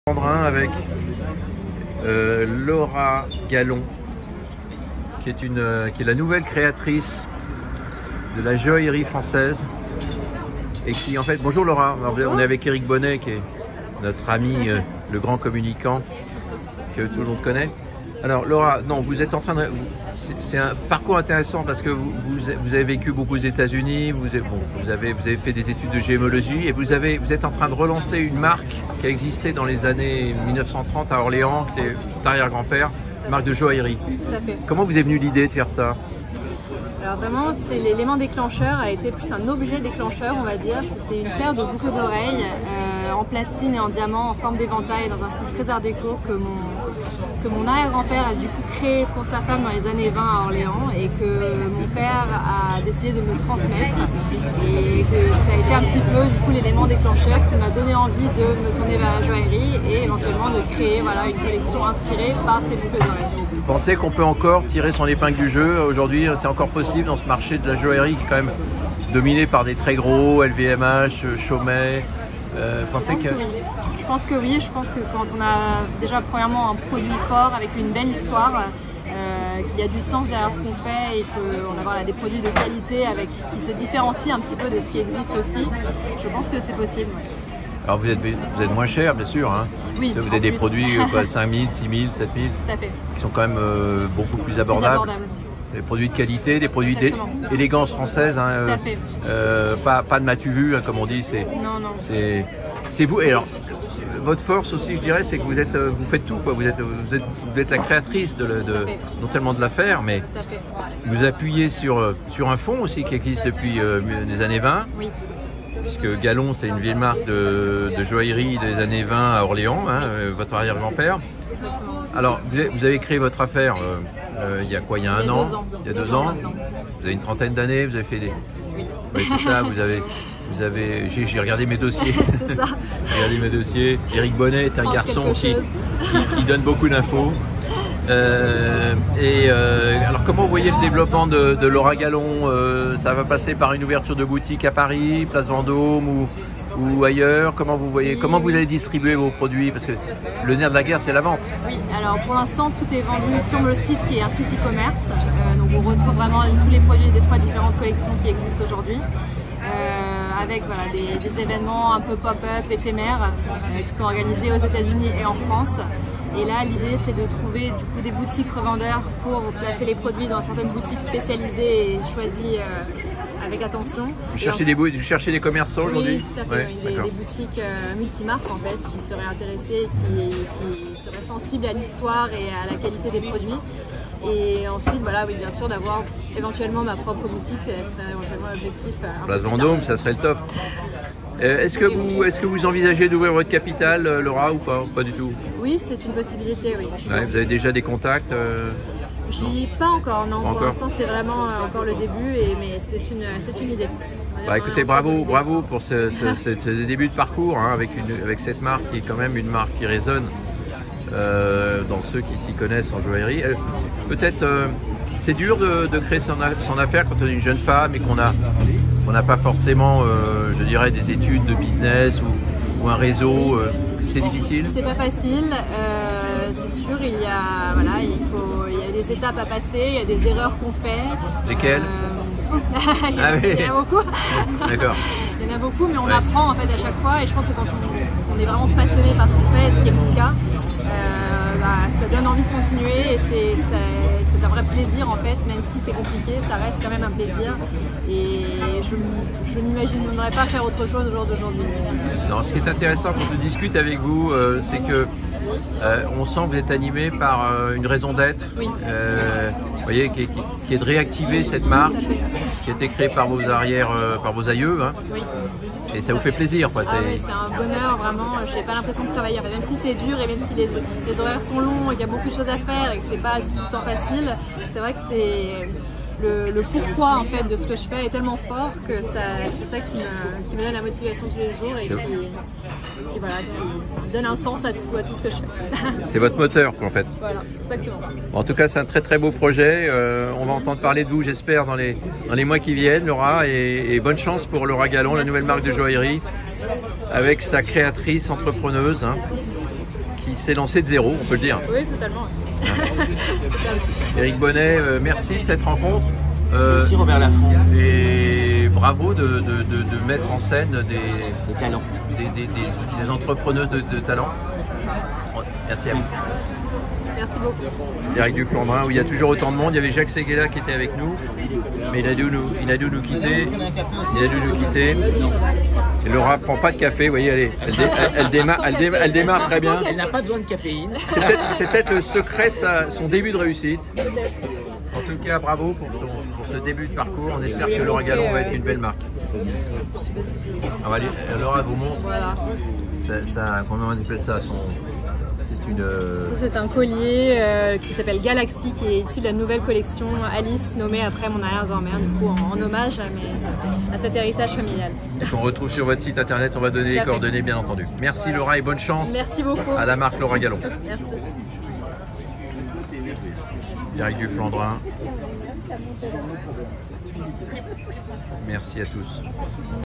Laura Gallon relance la marque de joaillerie de son arrière- grand père . Presqu’un conte de fées
En direct du Flandrin ,Paris XVI, Laura Gallon explique à Robert Lafont sa motivation à relancer la petite affaire de joaillerie fondée à Orléans dans les années 20 par son arrière grand-père . Une belle histoire que semble apprécier le communicant Éric Bonnet .